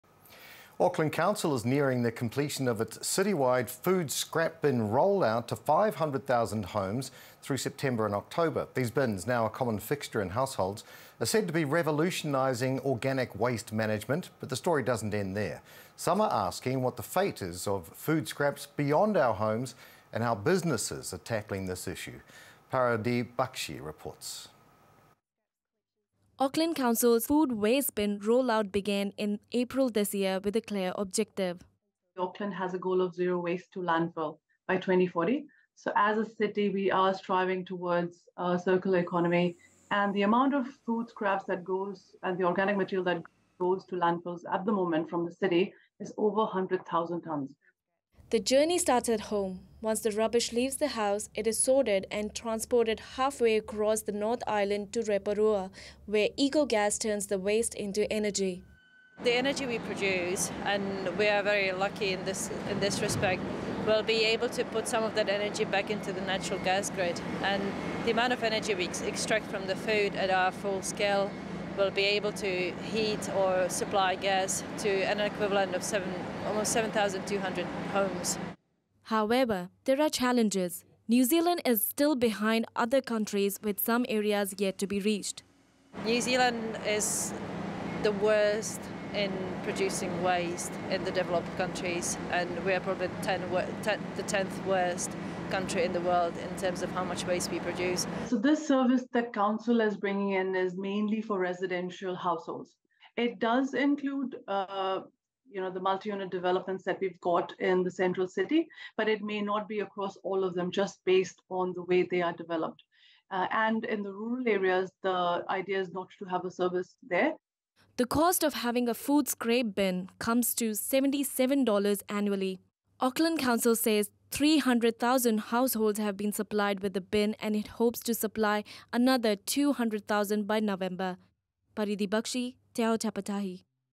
What is the fate of food scraps in Auckland Council's new food scrap bins?
Auckland Council's food scrap bin rollout to over 500,000 homes is nearing completion. It's being used to revolutionise organic waste management. But the question begs: where does it actually go?